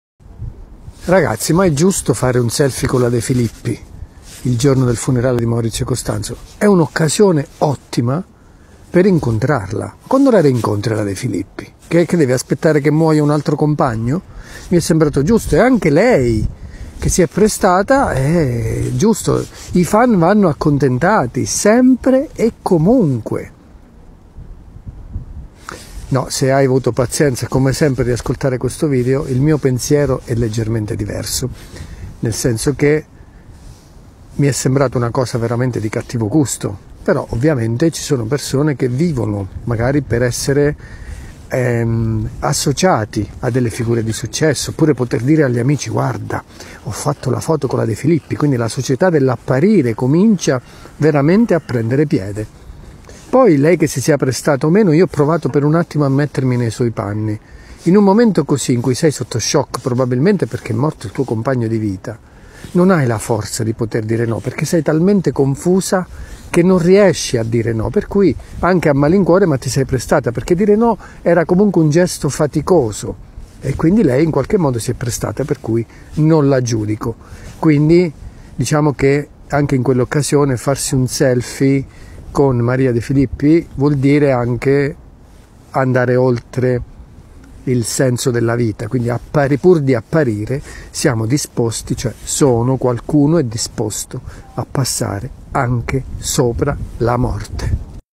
Un selfie con Maria De Fillippi!!!
Che meraviglia!! Finalmente ho fatto un selfie con Maria De Filippi!!!